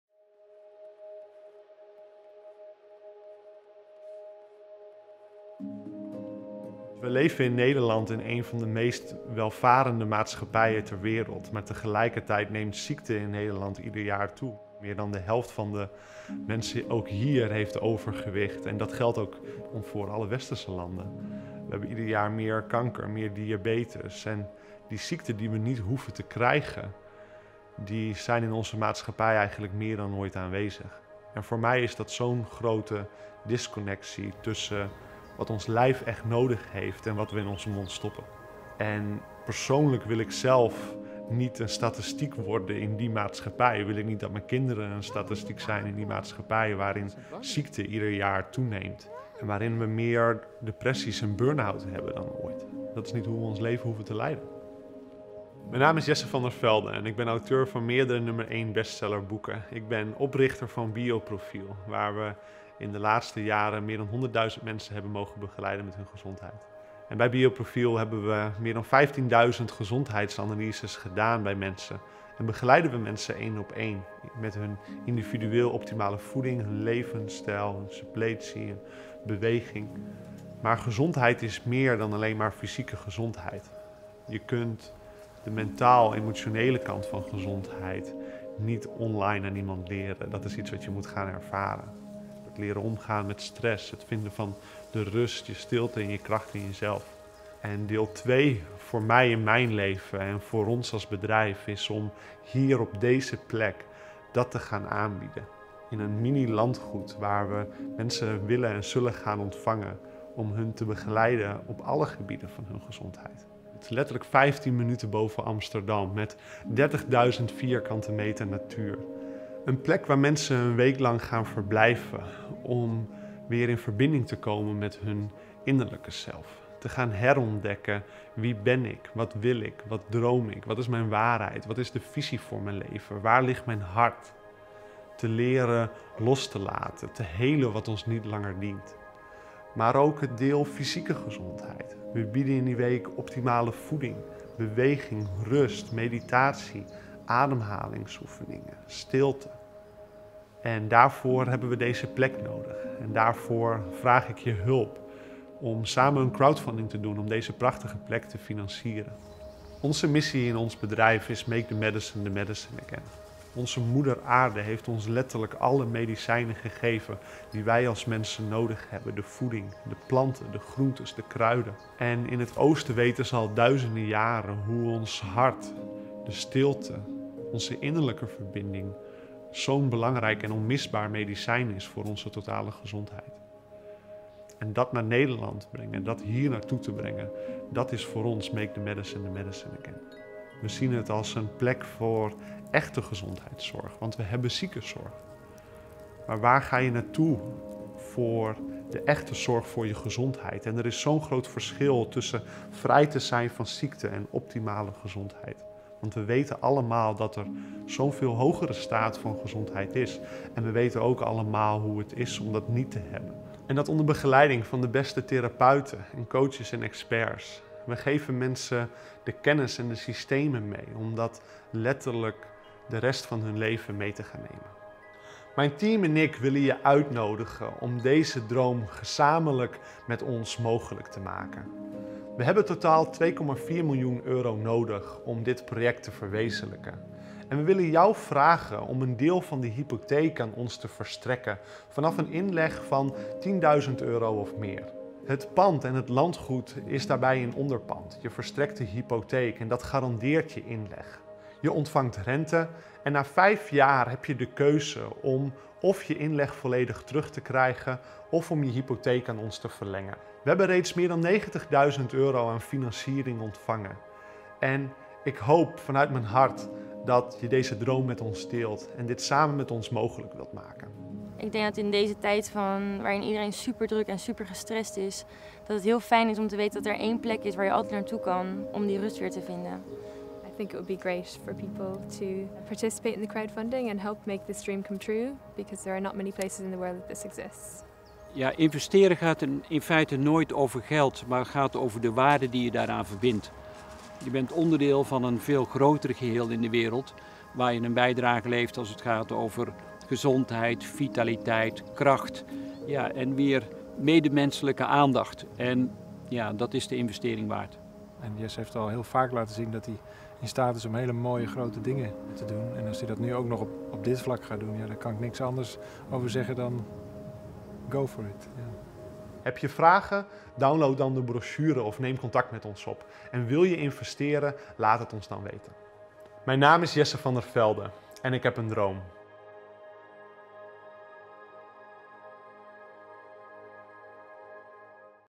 Crowdfunding: Creating a place of health & healing in Amsterdam, The Netherlands
In The Netherlands, we live in one of the most prosperous societies in the world.

Yet every year we see more disease. More than half of the population, even here, are overweight – and that is true of most Western countries.

Each year, there are more incidences of cancer, diabetes and medical conditions that we don’t have to get, which are more present than ever in our society.

For me, there is such a big disconnect in terms of what we need in our lives and what we put in our mouths.

My personal feeling is that I don’t want to become a statistic in this society. I don’t want my children to become statistics in a society where disease increases each year.

Where we have more cases of depression and burn-out than ever before. This is not how we have to live our lives.

My name is Jesse van der Velde and I am an author of many number 1 bestselling books.

I am the founder of Bioprofiel, where over the past few years, we have gone on a journey of health with more than 100 000 people.

More than 15 000 health analyses have been done at Bioprofiel. On a one-on-one basis, we have joined people in their search for more optimal nutrition, a better lifestyle, which supplements to take and activities to do.

But health is about more than just the physical.

You cannot teach someone online what the mental/emotional component is to good health, it is something that you have to experience for yourself.

Learning how to deal with stress, finding peace, silence and strength in yourself.

And part 2 of my life, and for us as a company, is to offer this here.

At a mini estate where we want to meet people and walk beside them every step of the way.

This place is only 15 minutes outside of Amsterdam and located in an area of 30 000 square metres of nature.

It is a place where people can come to stay for one week to again connect with their inner self.

To discover again: Who am I, what do I want, what do I dream about, what is my truth, my vision for my lie and where is my heart?

To learn to let go of that which no longer serves us.

But there is also the physical health side.

On a week here, we will offer optimal nutrition, movement, rest, meditation, breathing exercises, silence, and for that we need this place.

That is why I am asking for your help.

To help us with crowdfunding to finance all of this.

Our mission in our company is "Make the medicine, the medicine again"

Our Mother Earth has literally given us all of the medicine that we need – all the nutrition, the vegetables, the plants, the herbs.

In the East, people have known for thousands of years how our heart, silence and the inner connection are just as important and unmistakeable as medicine for our total health.

And bringing this to The Netherlands, and to this spot, is for us
"Make the medicine, the medicine again".

We see it as a place for really caring for your health, because we already have healthcare. But where do you go to really feel good? There is such a big difference between being free of disease and having optimal health.

We all know that such a purer state of health exists and we also know how it is not to be healthy.

This is what we want to offer, guided by the best therapists, coaches and experts. We will give people the knowledge and the systems that they can use for the rest of their lives.

My team and I would like to invite you to make this dream come true, with us.

We need a total of 2,4 million euro to realise this project.

And we would like to ask you to help us with the mortgage with an investment of 10.000 euro or more.

The property and the estate are the collateral. You take out a mortgage and this guarantees your investment.

You receive interest.

After 5 years you have a choice: receive back all of your investment or extend your mortgage with us.

We have already received more than 90 000 euro in financing.

I hope with all of my heart that you will share this dream with us and want to make it come true, together.

“I think that in these times, when people are under pressure and super stressed, it’s great that there is always a place where you can go to, to find peace again.”

- English testimonial –

"Investing actually is never just about money. It is about the value that you associate with it. You are a part of a much bigger picture, where you can make a contribution to better health, vitality, strength and human consciousness. And that is worth the investment”

"Jesse has often shown that he is capable of doing beautiful, big things and if he is planning something like this, then I can say nothing but: "Go for it".

Would you like to invest? Let us know.

My name is Jesse van der Velde and I have a dream.